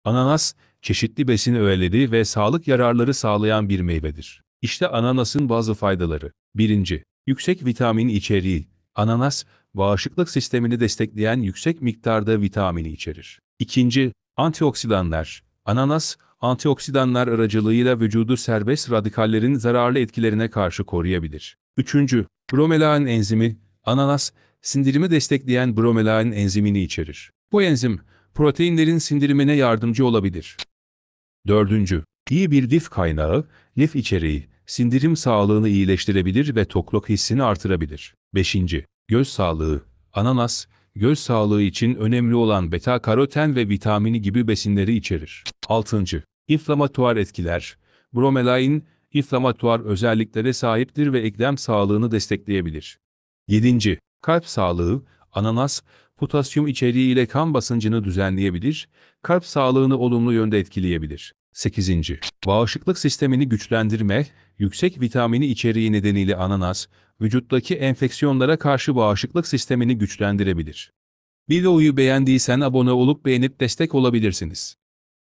Ananasın faydaları nelerdir?
Ananasın faydaları nelerdir? | Sağlıklı yaşam ve beslenme.

Ananas, çeşitli besin öğeleri ve sağlık yararları sağlayan bir meyvedir. İşte ananasın bazı faydaları:

1. **Yüksek C Vitamini İçeriği:** Ananas, bağışıklık sistemini destekleyen yüksek miktarda C vitamini içerir.
2. **Antioksidanlar:** Ananas, antioksidanlar aracılığıyla vücudu serbest radikallerin zararlı etkilerine karşı koruyabilir.
3. **Bromelain Enzimi:** Ananas, sindirimi destekleyen bromelain enzimini içerir. Bu enzim, proteinlerin sindirimine yardımcı olabilir.
4. **İyi Bir Lif Kaynağı:** Lif içeriği, sindirim sağlığını iyileştirebilir ve tokluk hissini artırabilir.
5. **Göz Sağlığı:** Ananas, göz sağlığı için önemli olan beta-karoten ve C vitamini gibi besinleri içerir.
6. **Anti-İnflamatuar Etkiler:** Bromelain, anti-inflamatuar özelliklere sahiptir ve eklem sağlığını destekleyebilir.
7. **Kalp Sağlığı:** Ananas, potasyum içeriği ile kan basıncını düzenleyebilir, kalp sağlığını olumlu yönde etkileyebilir.
8. **Bağışıklık Sistemini Güçlendirme:** Yüksek C vitamini içeriği nedeniyle ananas, vücuttaki enfeksiyonlara karşı bağışıklık sistemini güçlendirebilir.
Ancak, herhangi bir diyet değişikliği gibi, ananas tüketiminde aşırıya kaçılmaması önemlidir. Sağlık durumunuz ve kişisel ihtiyaçlarınız hakkında doktorunuzla konuşmak da faydalı olabilir.